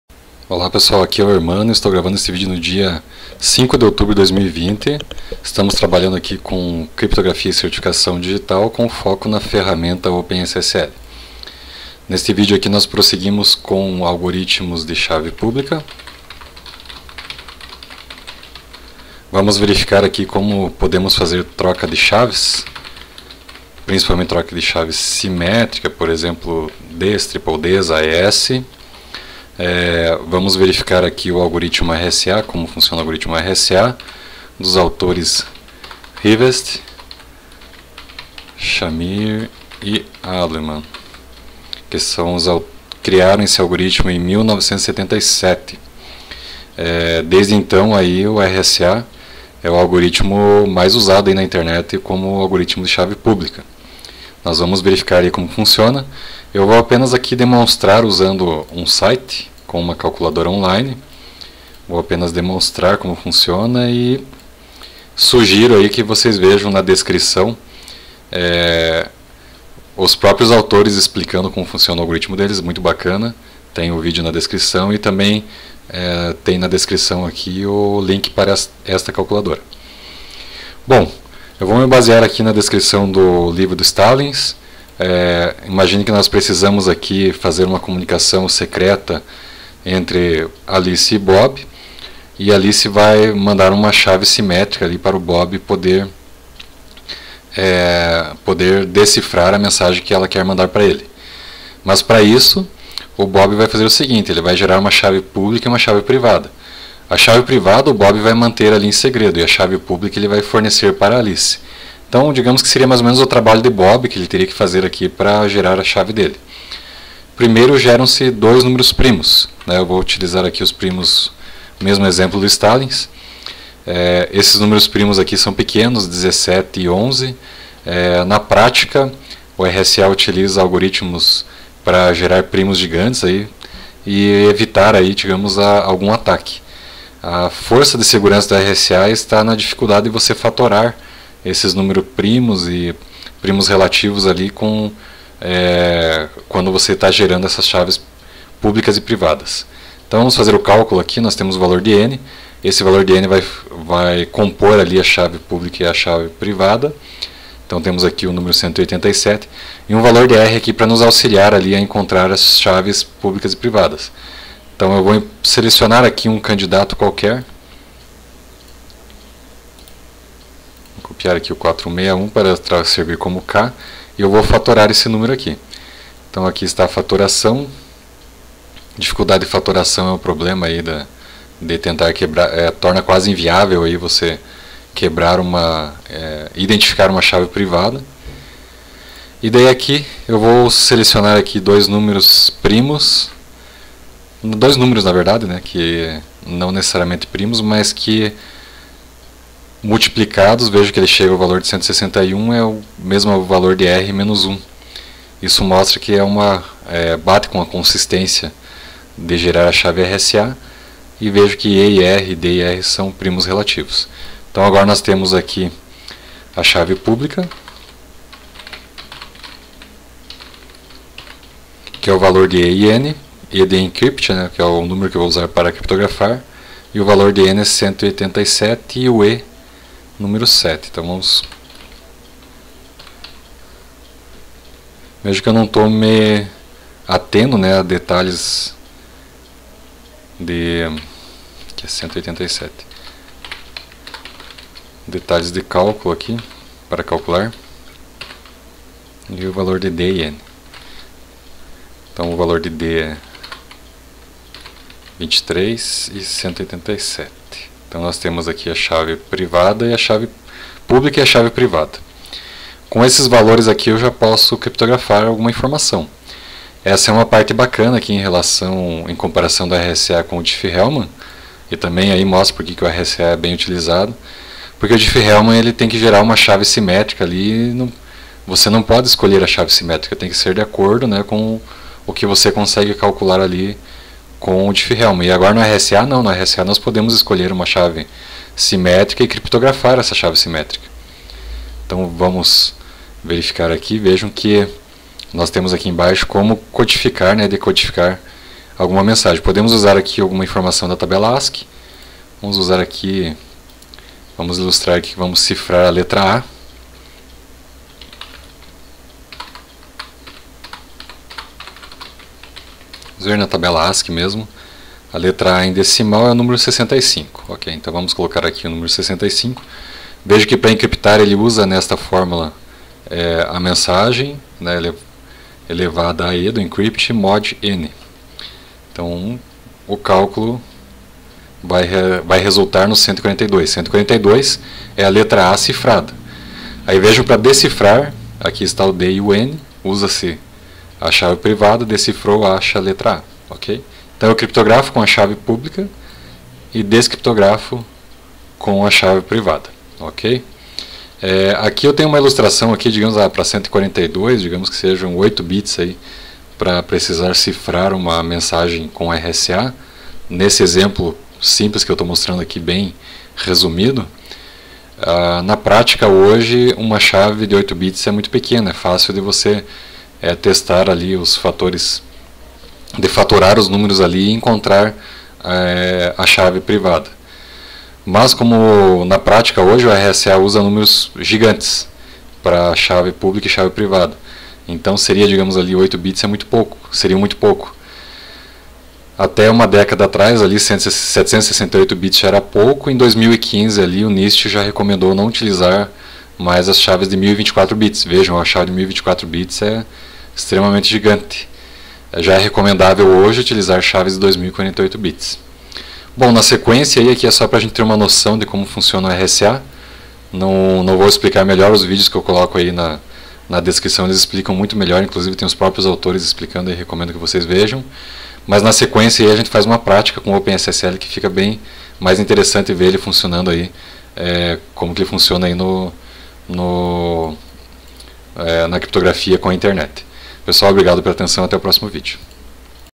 Cripto e Cert. Digital - APNP 10a - Troca de Chaves - RSA (parte 1)
Capa:
Universidade Tecnológica Federal do Paraná - UTFPR
Câmpus Guarapuava - Paraná
Curso de Tecnologia em Sistemas para Internet - TSI
Tópicos Especiais em Computação
Criptografia e Certificação Digital
Atividades Pedagógicas Não Presenciais - Aula 10a
Título: Troca de Chaves - RSA (parte 1)
Prof. Hermano Pereira
Em 05/10/2020

-x-x-x-x-x-x-x-x-

Livros:

[STALLINGS 2008] Stallings, W.; Criptografia e segurança de redes: princípios e práticas - 4a Edição: Prentice-Hall, 2008.
- Cap. 9 - Criptografia de chave pública RSA
- Cap. 9.1 - Algoritmo RSA
- Cap. 10.2 - Acordo de chaves Diffie-Hellman
- Cap. 10.3-4 - Algoritmo ECC

[Forouzan 2008] Forouzan, B.A.; Comunicação de Dados e Redes de Computadores - Tradução da 4a Edição - McGraw-Hill, 2008.
- Cap. 30.3 - RSA e Diffie-Hellman

[Tanenbaum 2011] Tanenbaum, A.S.; Wetherall, D.; Redes de Computadores - Tradução da 5a Edição - Prentice Hall, 2011.
- Cap 8.3.1 - Algoritmos: RSA

[Kurose 2010] Kurose, J.F.; Ross,K.W.; Redes de Computadores e a Internet: uma abordagem top-down - Tradução da 5a Edição - Pearson Education do Brasil, 2010.
- Cap. 8.2.2 - Algoritmos: RSA

DSS Digital Signature Standard - FIPS 186-4

Code.org The Internet: Encryption & Public Keys

RSA Conference Rivest, Shamir, Adleman - The RSA Algorithm Explained

Se encontrarem mais vídeos e documentários, favor compartilhar.